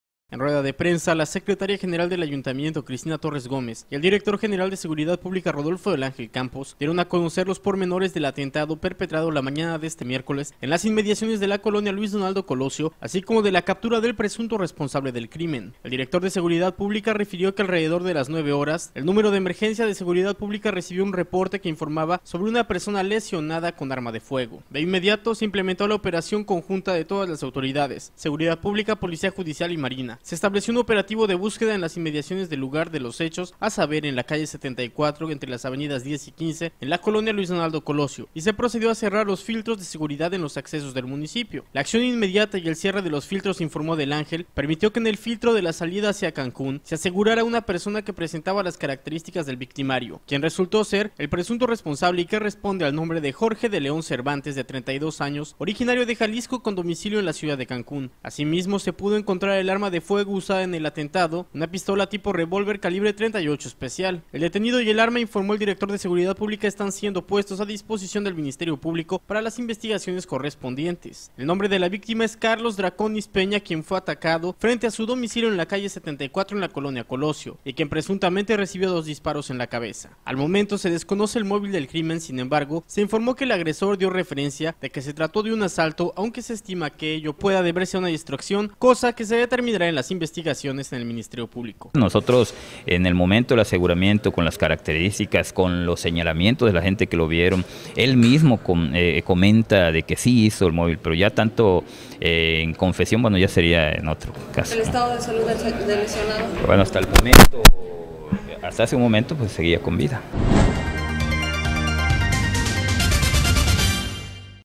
Capturan a presunto sicario
Playa del Carmen, Q. Roo.-En rueda de prensa, la secretaria general del ayuntamiento Cristina Torres Gómez y el director general de Seguridad Rodolfo del Ángel Campos, dieron a conocer los pormenores del atentado perpetrado la mañana de este miércoles en las inmediaciones de la colonia Colosio así como de la captura del presunto responsable del crimen.